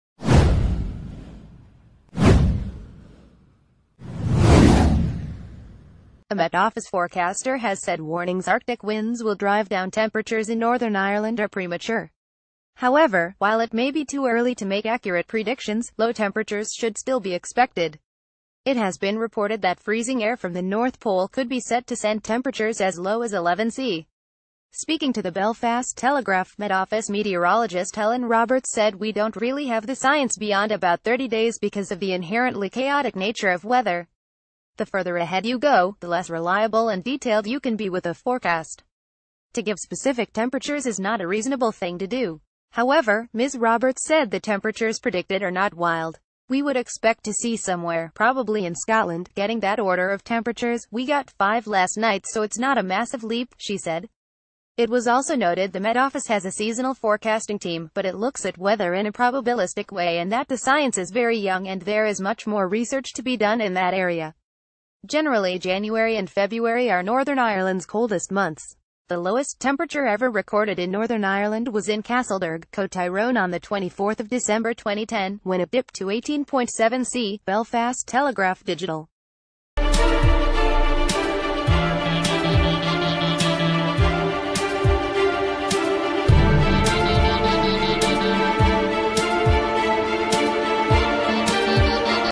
Breaking News | Arctic winds warning for northern ireland 'premature' but low temperatures possible
Arctic winds warning for northern ireland 'premature' but low temperatures possible, says met office

A Met Office forecaster has said warnings arctic winds will drive down temperatures in Northern Irel...